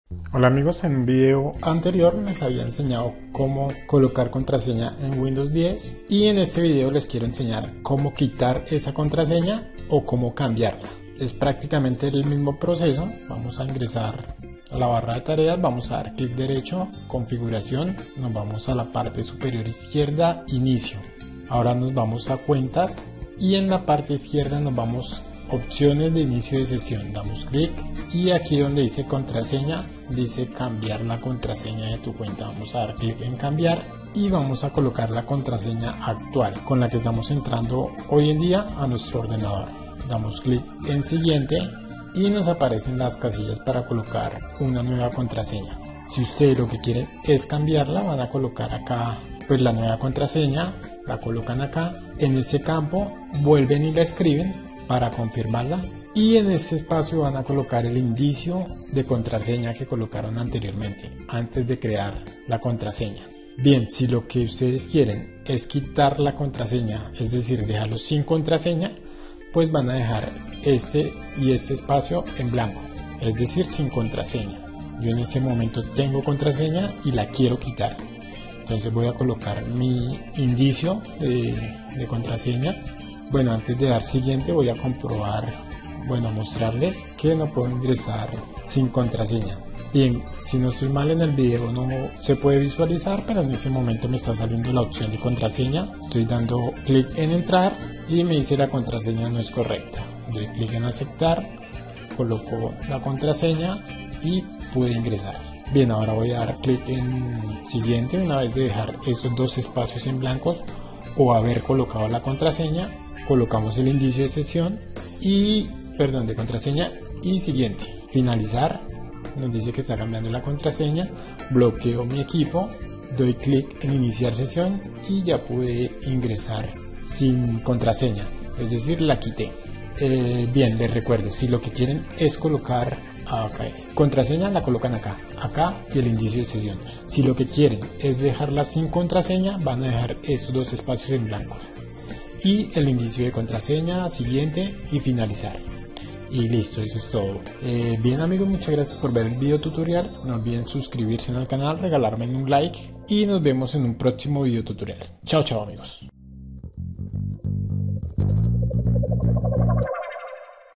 🔐 como CAMBIAR LA CONTRASEÑA de inicio de sesión EN WINDOWS 10
como cambiar o quitar la contraseña en windows 10

hola amigos hoy vamos a quitar la contraseña en windows 10 para poder iniciar sesión sin necesidad de colocar la contraseña, y también veremos como cambiar la contraseña en windows 10  en caso que sospechamos que otra persona ya sabe la contraseña de inicio de sesión bien aunque los dos pasos son muy similares primero veremos como quitar la contraseña para iniciar sesión para esto debemos ir a la barra de tareas dar clic derecho configuración y en la ventana que no sale nos vamos a la parte superior izquierda y damos clic en inicio ahora seleccionamos la opción cuentas y damos clic sobre opciones de inicio de sesión en parte izquierda ahora buscamos donde dice contraseña y damos clic en agregar bien para quitar la contraseña en windows 10 vamos a dejar los dos primeros espacios en blanco es decir sin contraseña y en la tercera opción colocamos el indicio de sesión que colocamos cuando creamos la contraseña y por último vamos a dar clic en siguiente y listo ya vamos a poder iniciar sesión sin contraseña es decir ya quitaremos la contraseña en windows 10 ahora para cambiarla vamos hacer los mismos pasos pero cuando lleguemos a la opción agregar contraseña en los espacios 1 y 2 vamos a colocar la nueva contraseña que deseamos para iniciar sesión en windows 10 y en el 3 campo vamos a colocar el indicio de contraseña y por ultimo siguiente y listo nuestro windows 10 va quedar con una nueva contraseña para iniciar sesión.

Capítulos
0:00 Presentación
0:12 cambiar o quitar la contraseña de inicio de sesión en windows 10
2:27 Agradecimientos